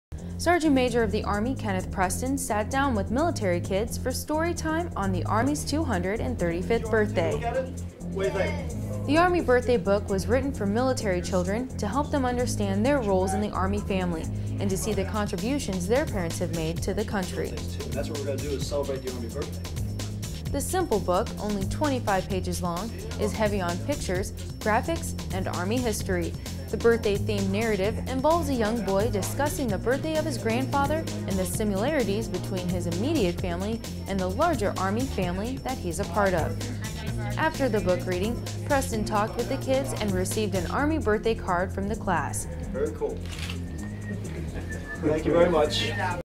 Story time for the Army Birthday
Sgt. Maj. of the Army Kenneth O. Preston sat down with military children to read to them the Army Birthday book. Sgt. Ashleigh Torres reports.